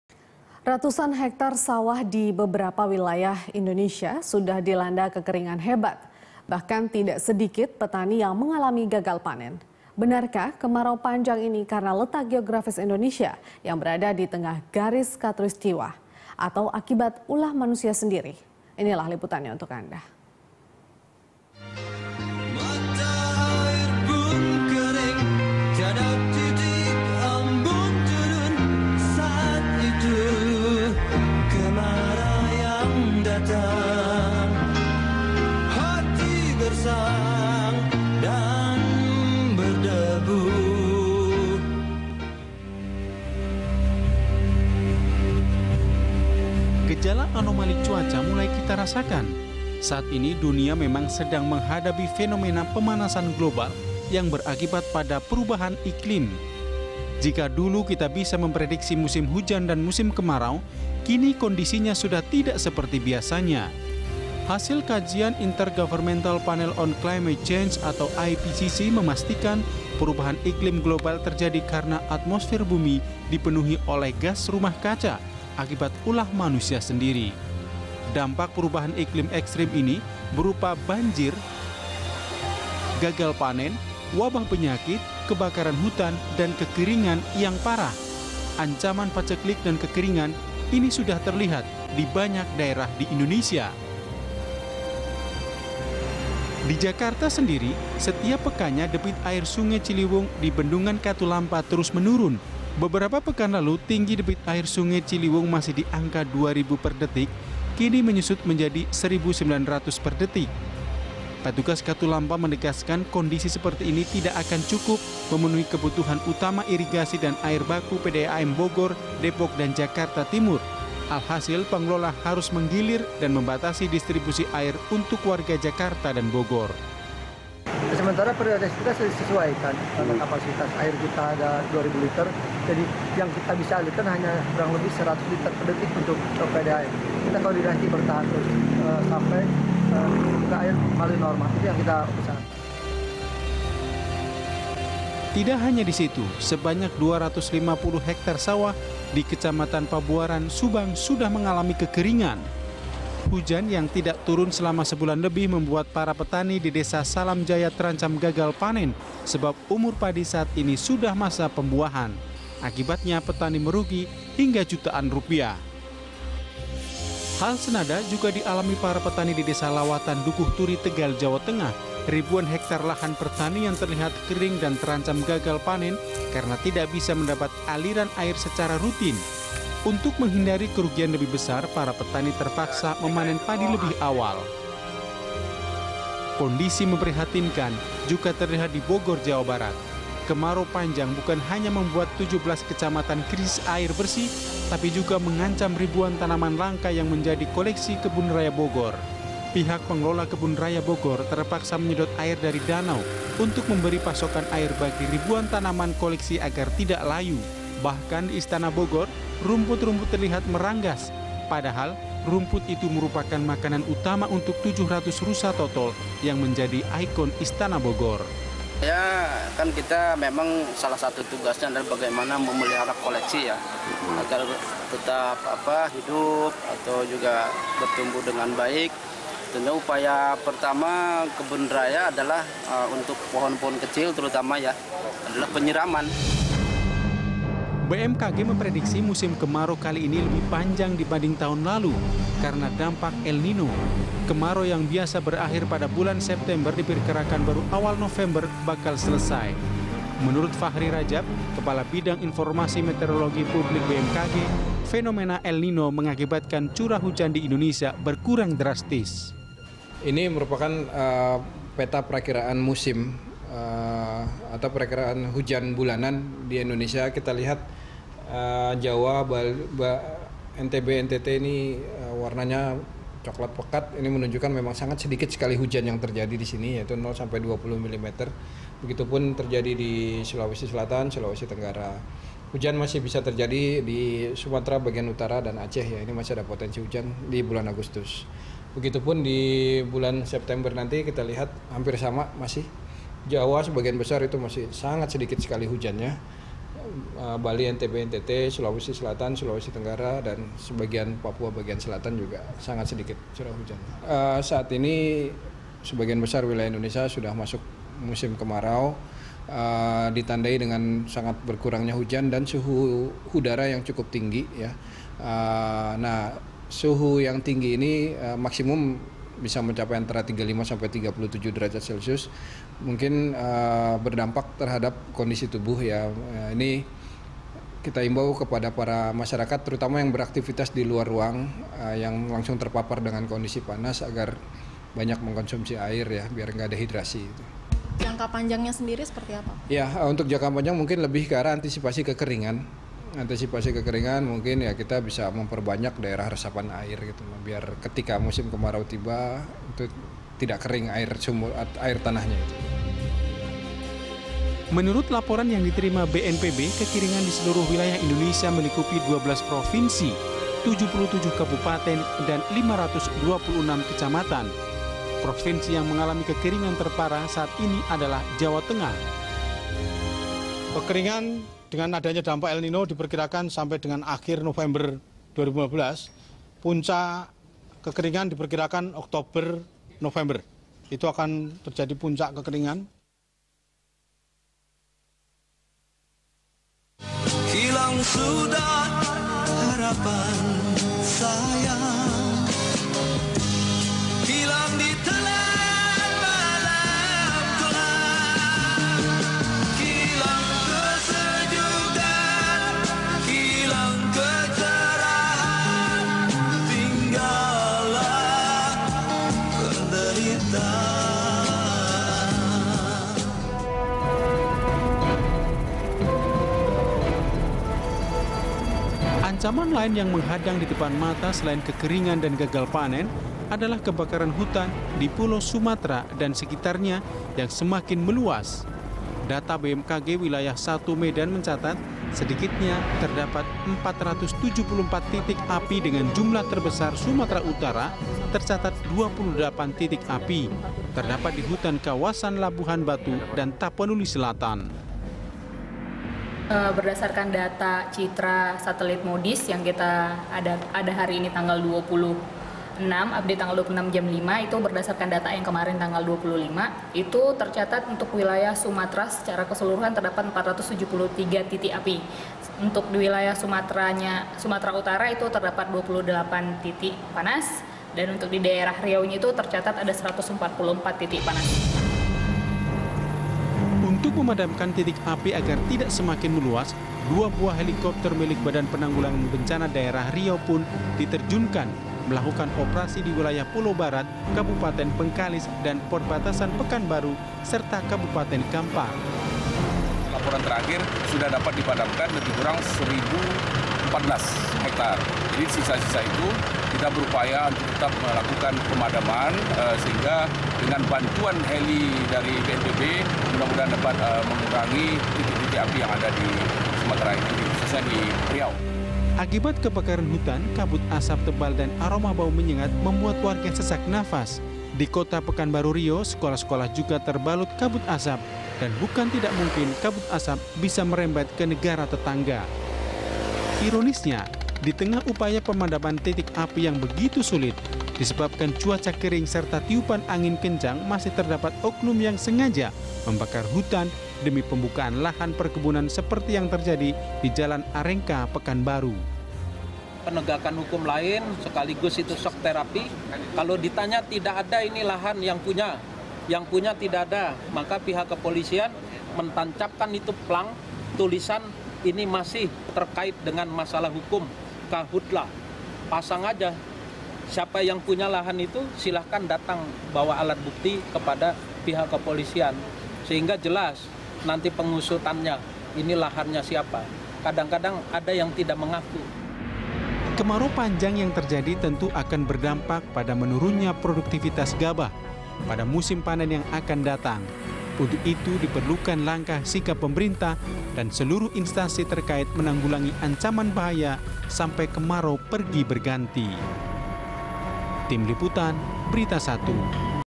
News of The Week: Kemarau Panjang Landa Negeri
Ratusan hektar sawah di beberapa wilayah Indonesia sudah dilanda kekeringan hebat. Bahkan tidak sedikit petani yang mengalami gagal panen.

Facebook.com/BeritaSatuTV
Youtube.com/BeritaSatu